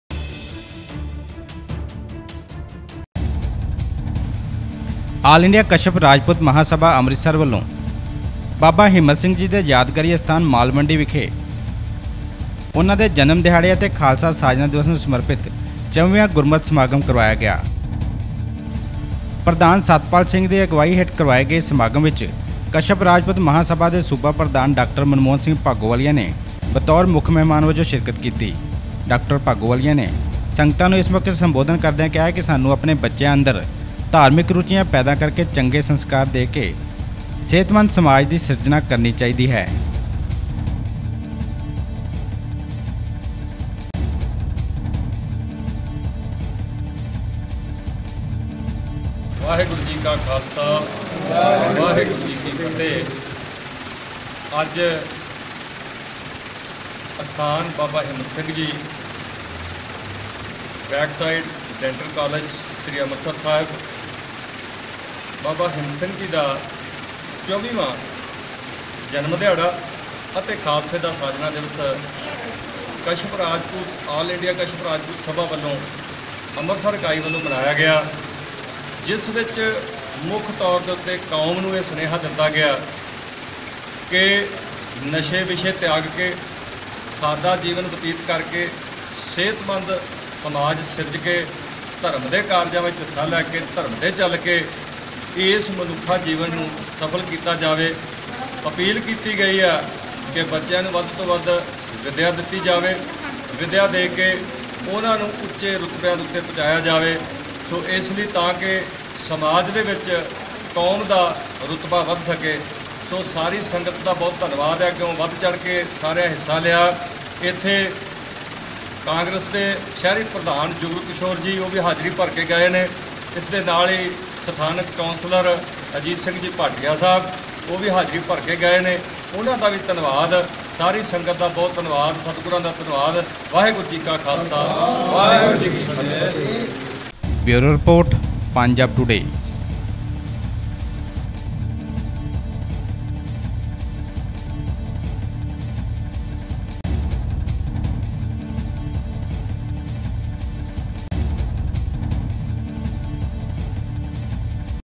ਆਲ ਇੰਡੀਆ ਕਸ਼ਯਪ ਰਾਜਪੂਤ ਸਭਾ ਧਾਰਮਿਕ ਸਮਾਗਮ,Dr manmohan singh Bhagowalia
ਆਲ ਇੰਡੀਆ ਕਸ਼ਯਪ ਰਾਜਪੂਤ ਸਭਾ ਧਾਰਮਿਕ ਸਮਾਗਮ
ਬਾਬਾ ਹਿੰਮਤ ਸਿੰਘ ਦੇ ਜਨਮ ਦਿਨ ਨੂੰ ਸਮਰਪਿਤ ਸਮਾਗਮ ਚ